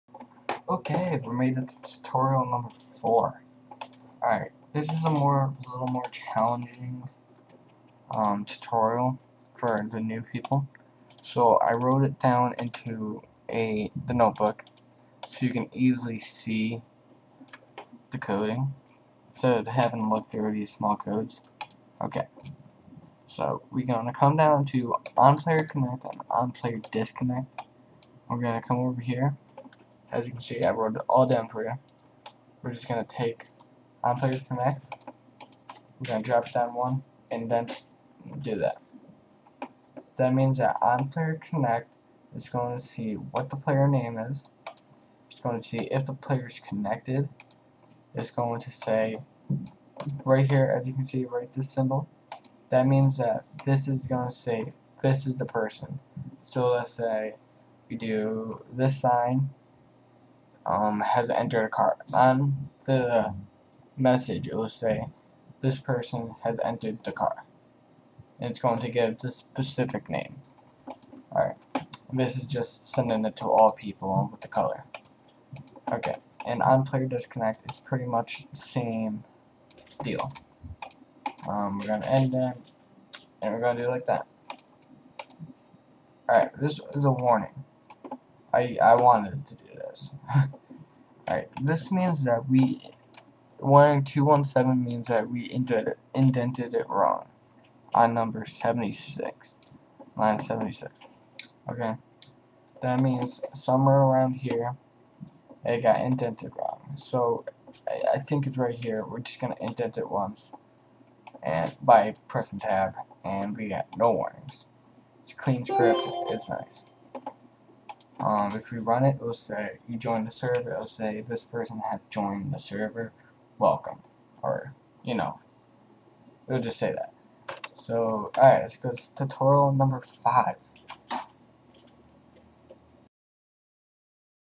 SA-MP Tutorial- Adding a Joining Message And Disconnecting Message
SA-MP Tutorial 5- Adding a Joining Message And Disconnecting Message

it Will Say This Player Has Joined The Game Or This Person Has Left The Game!